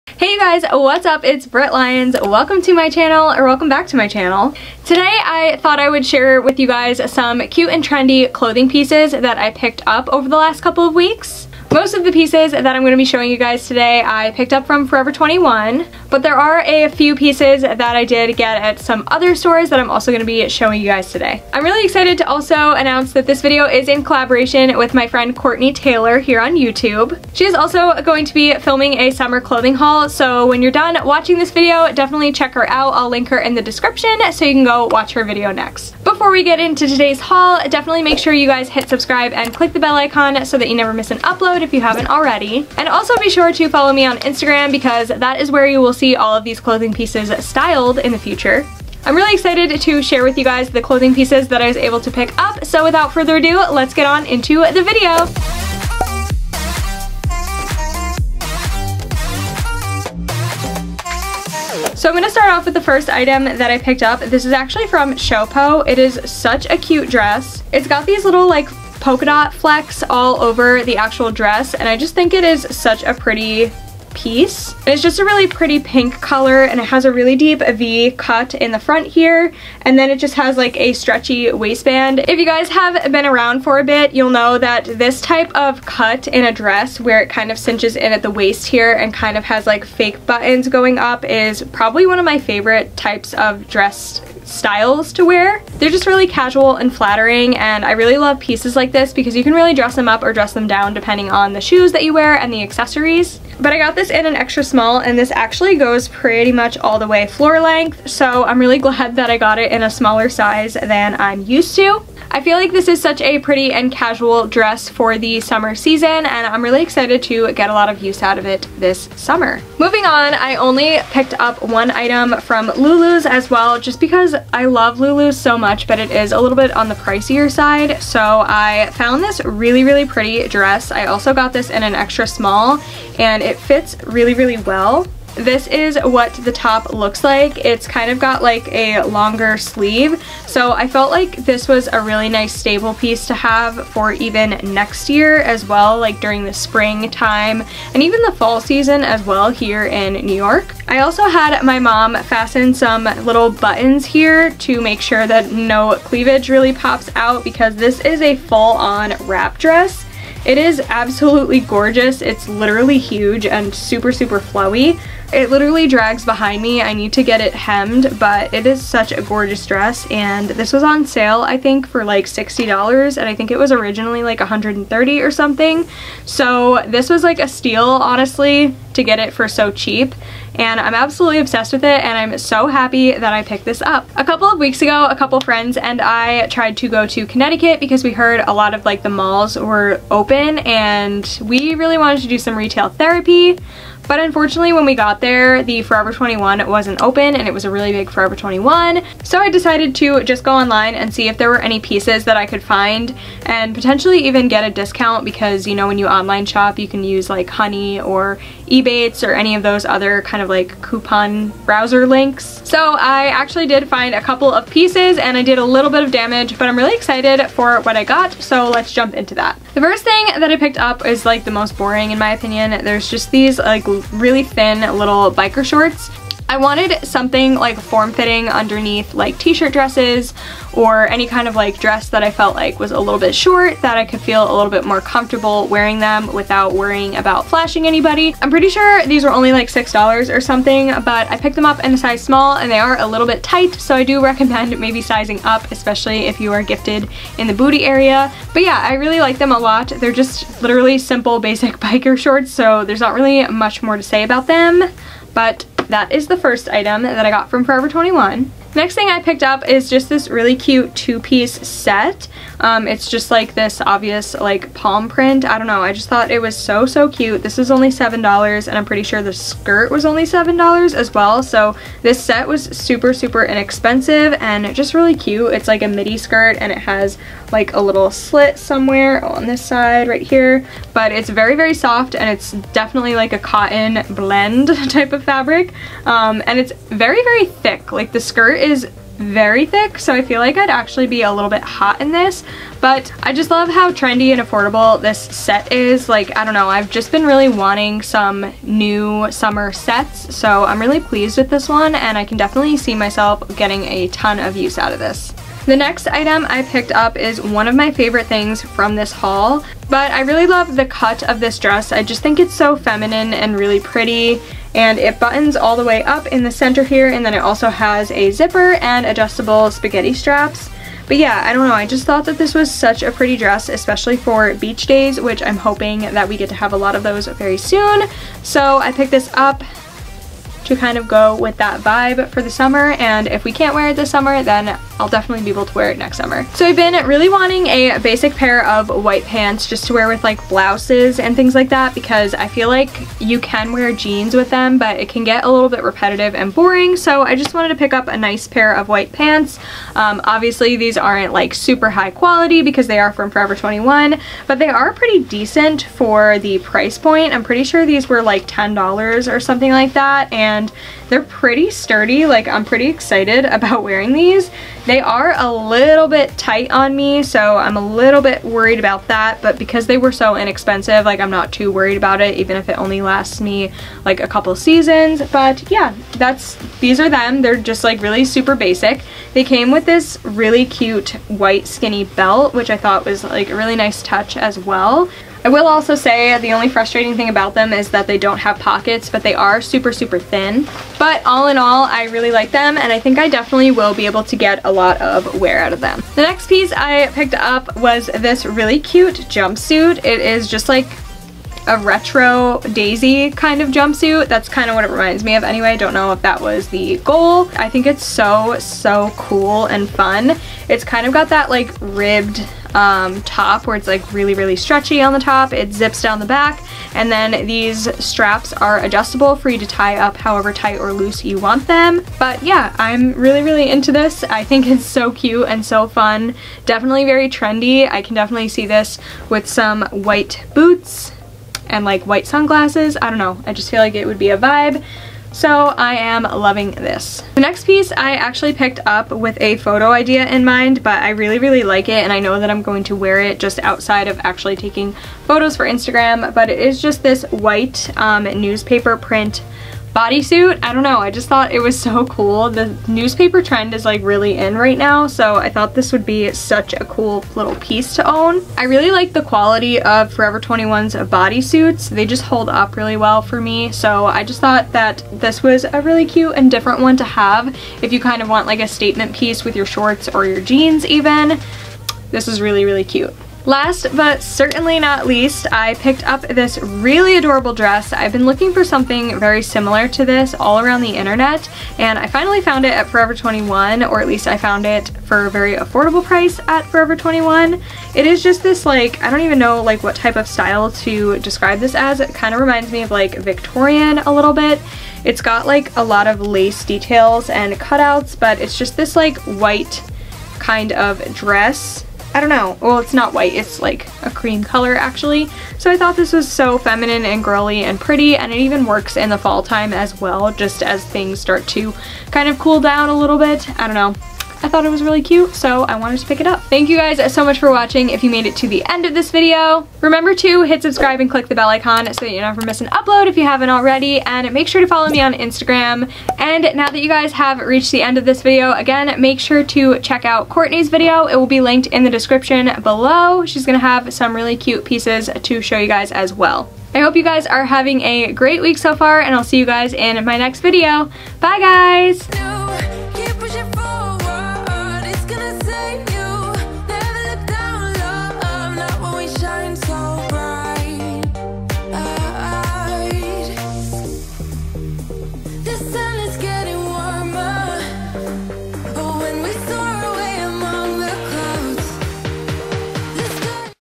Instagram Inspired Summer Try On Haul
Hey ya'll!

Today I wanted to share with you a trendy Instagram inspired summer try on haul! I hope you guys enjoy this video!

This video is in collaboration with my friend Courtney Taylor here on Youtube!
⋆ Courtney's IG: @CourtneyTayyyyy

Xoxo,
Britt

⋒ MY EVERYDAY MAKEUP ⋒
Eyebrow Pomade

⋒ HAIR ⋒
Hair Curler
Leave-in Conditioner
Dry Shampoo

Vlog Camera
Main Camera